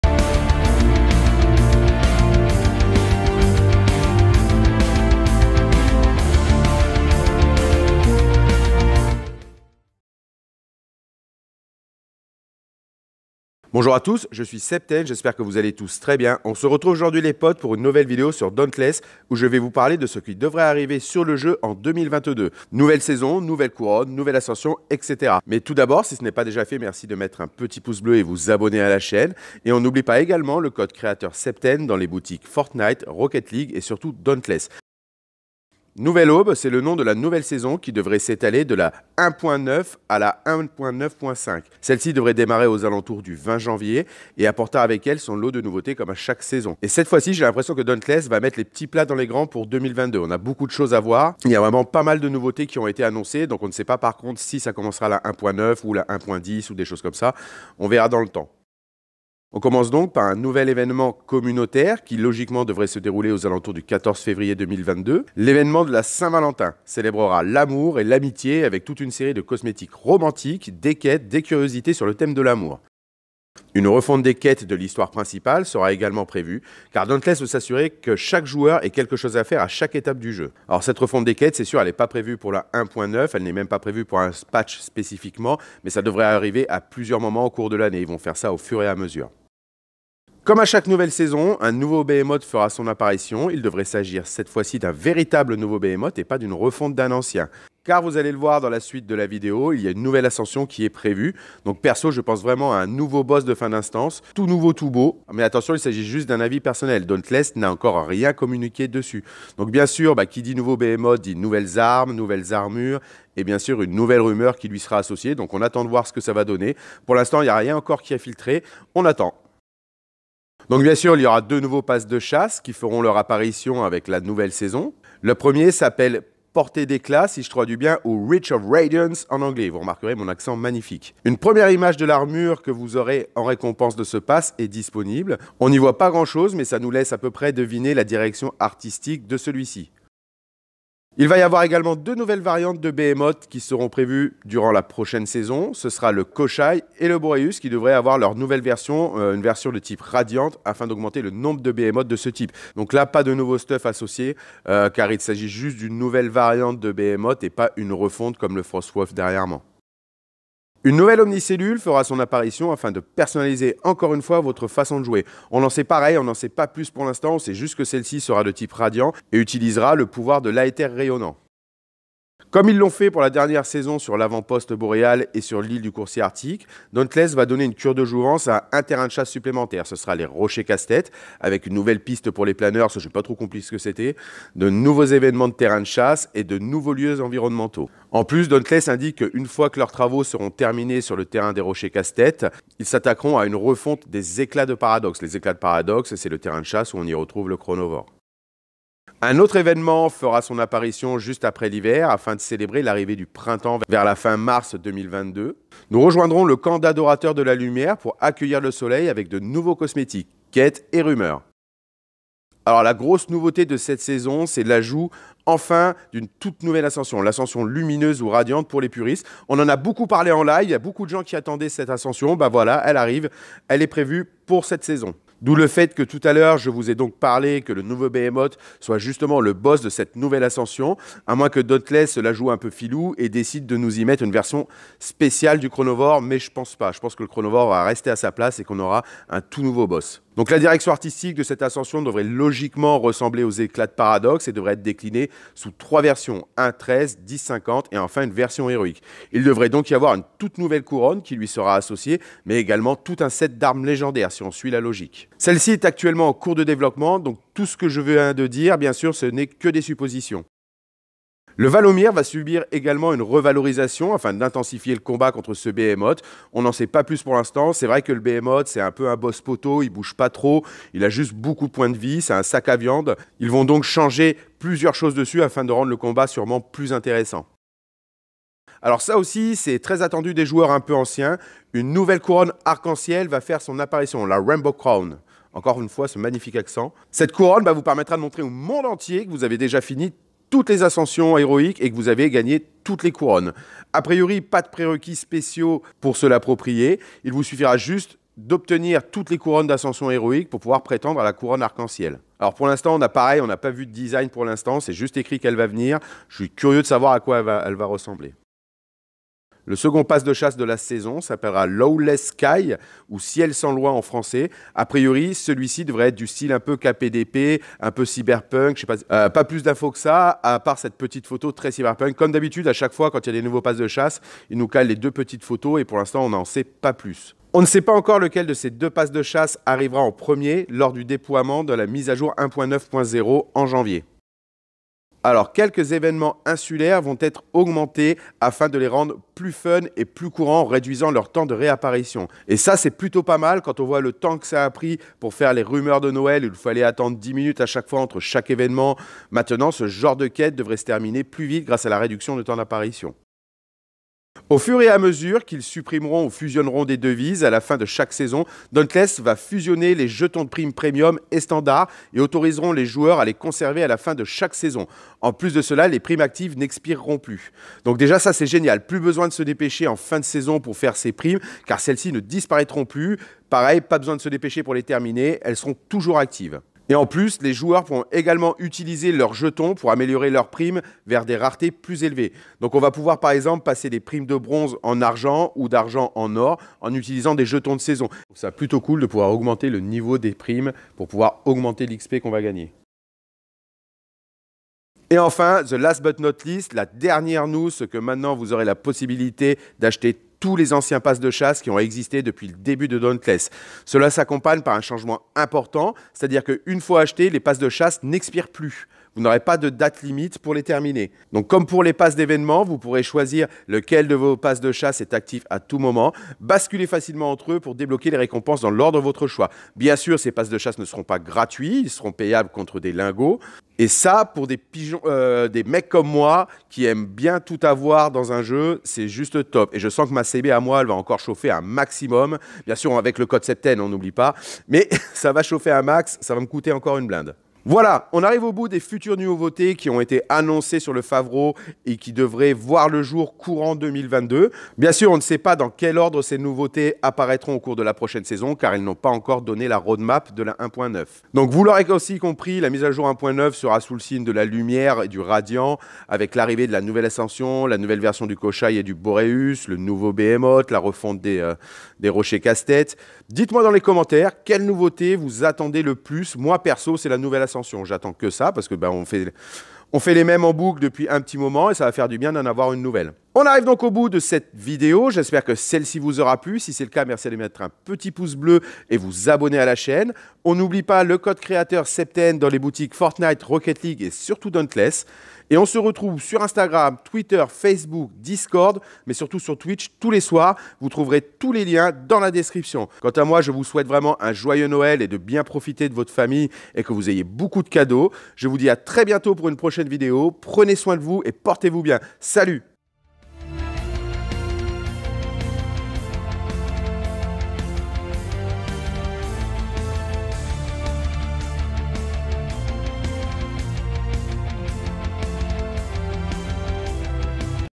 PROCHAINE SAISON ET NOUVEAUTÉS 2022 SUR DAUNTLESS
☑️ Nouvelle vidéo sur Dauntless : PROCHAINE SAISON ET NOUVEAUTÉS 2022 SUR DAUNTLESS
Petite vidéo sur les nouveautés 2022
N'hésite pas à venir me voir en Stream sur Twitch ici ☑️